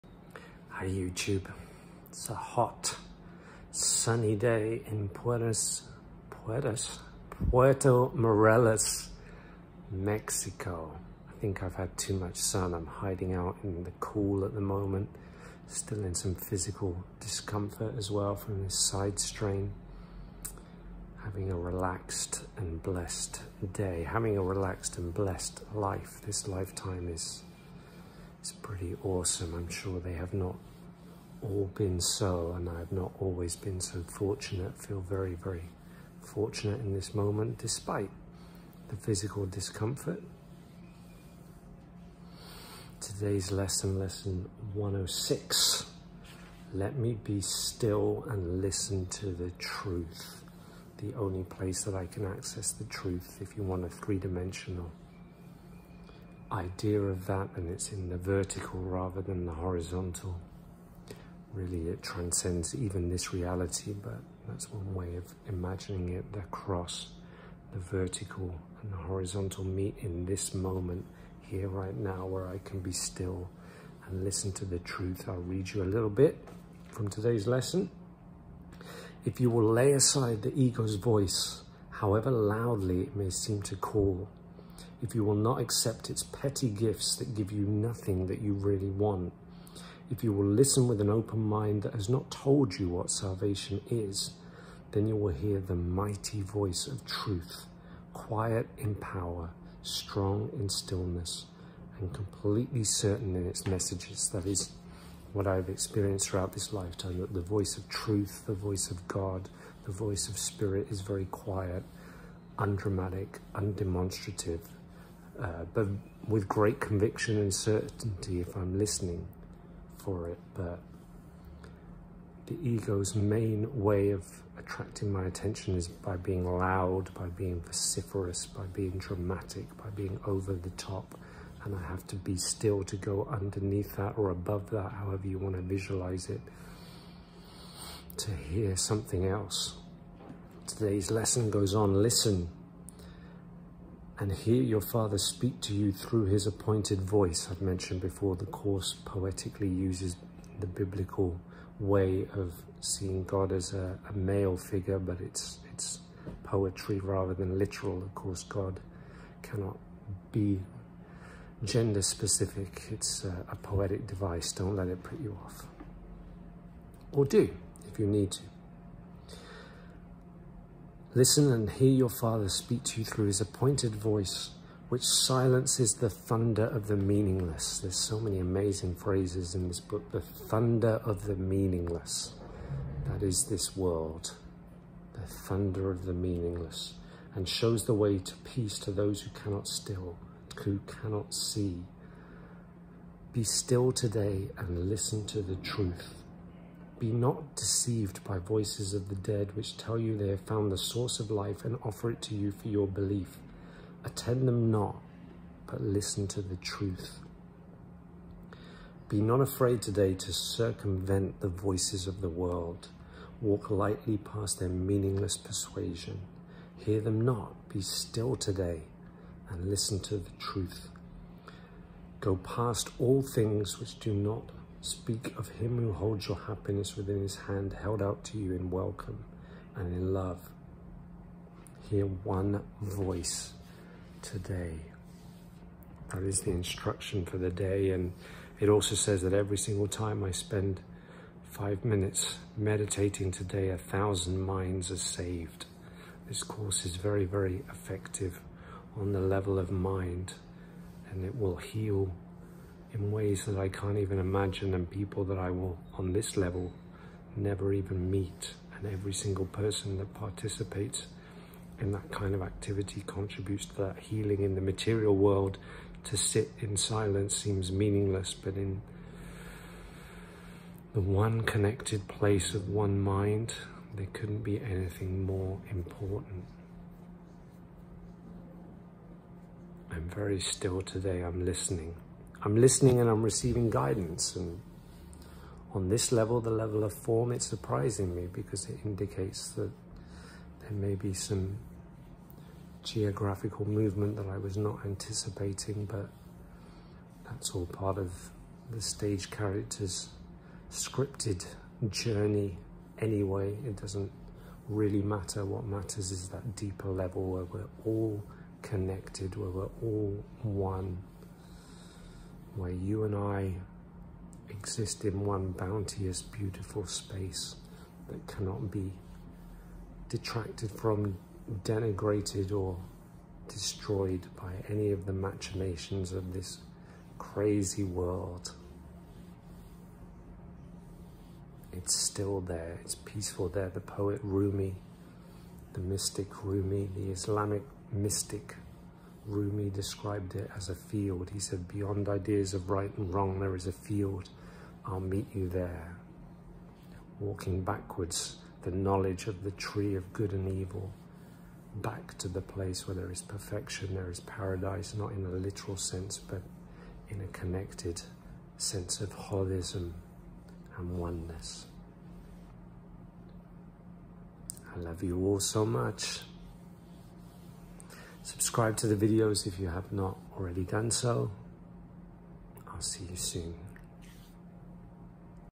A Course In Miracles Lesson 106: Let me be still and listen to the truth.
Lesson 106 of the Workbook of A Course In Miracles is Let me be still and listen to the truth.

Final night in beautiful Puerto Morelos, Mexico